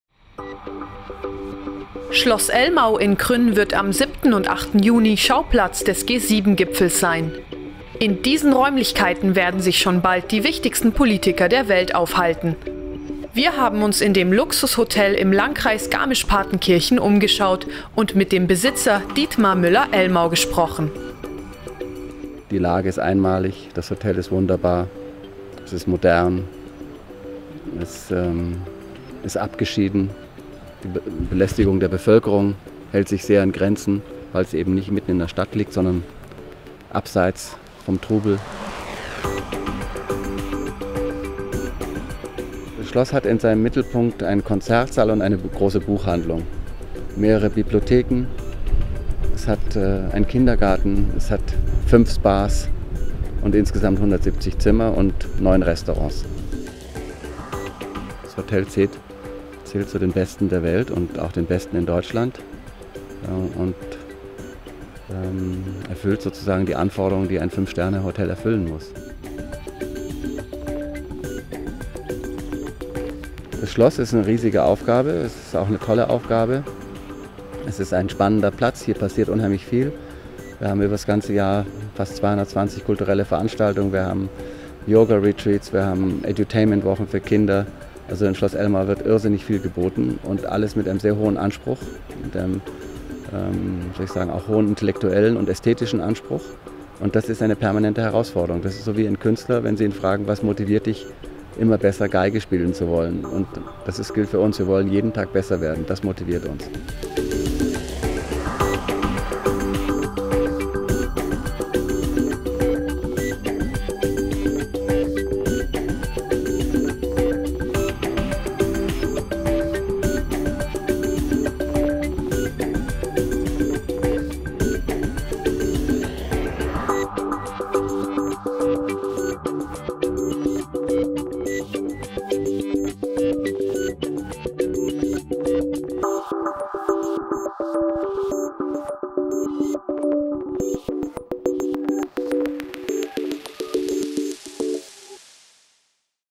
G7-Gipfel in Elmau: So sieht das Schloss von Innen aus
Krün/Garmisch-Partenkirchen/München - Der G7-Gipfel auf Schloss Elmau steht kurz bevor. Wir haben uns vorher noch im Schloss umgeschaut und mit dem Schloss-Herrn Dietmar Müller-Elmau gesprochen.